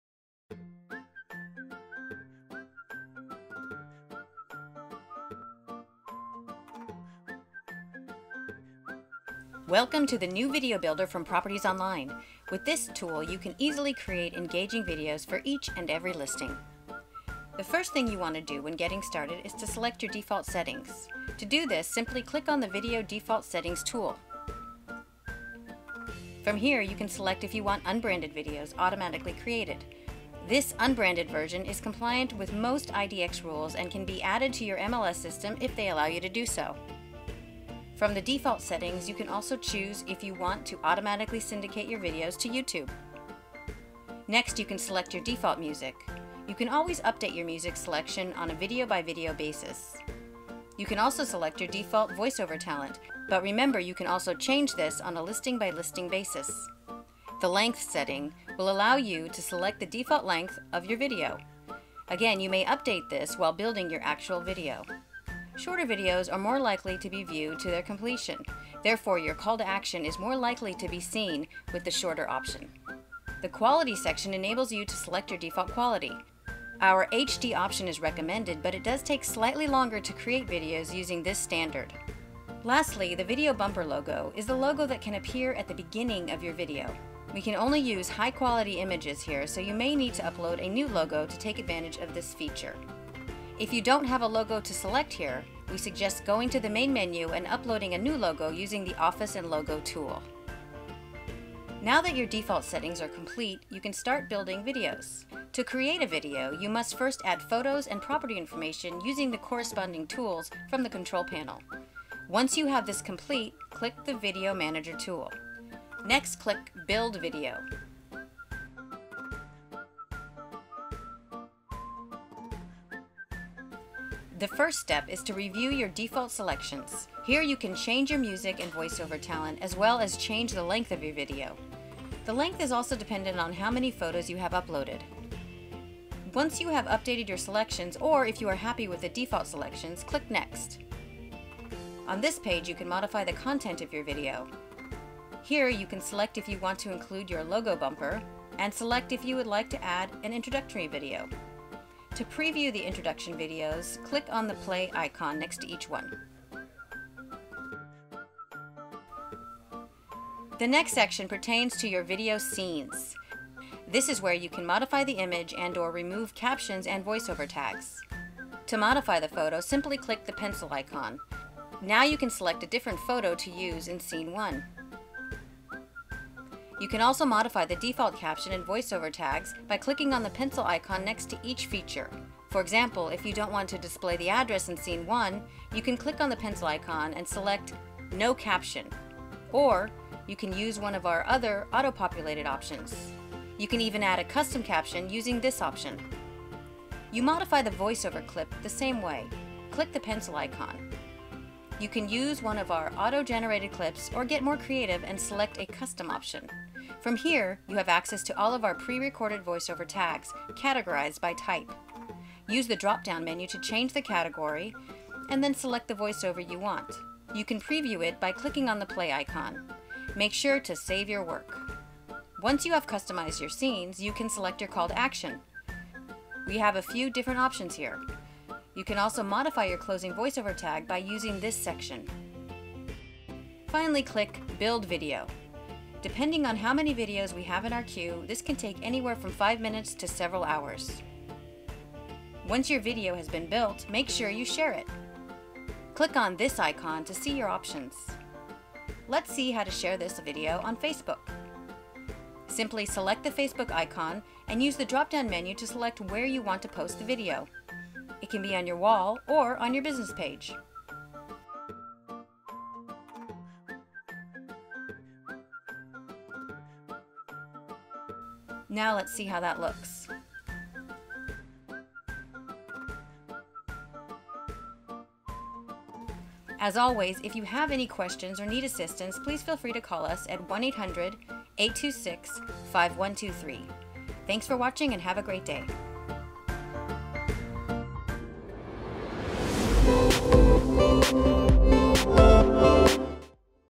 Video Builder
How to create a video using the new video builder from Properties Online.